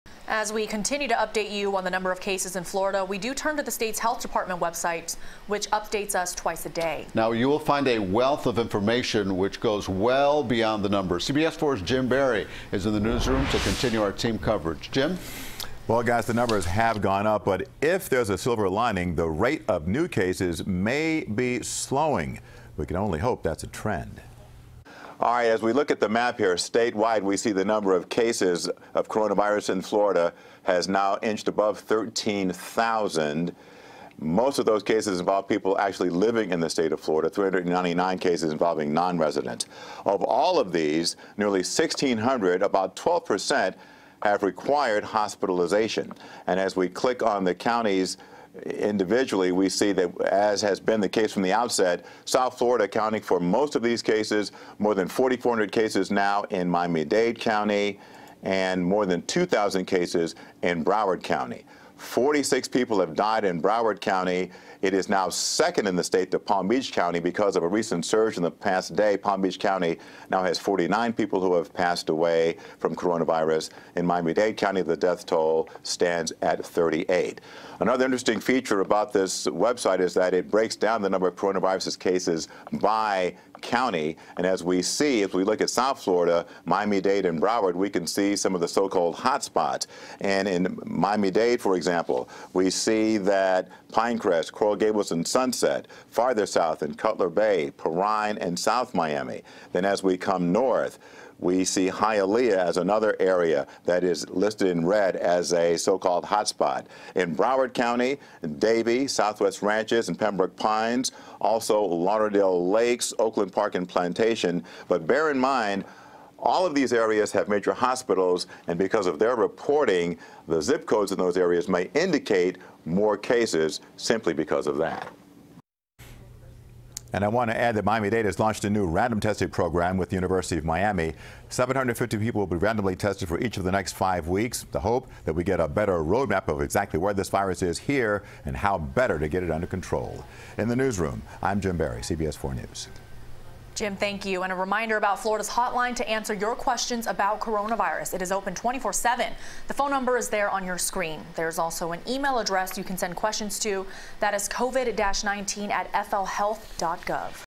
Coronavirus Numbers Deep Dive 4-6-20
CBS4's Jim Berry breaks down the Florida Department of Health's dashboard.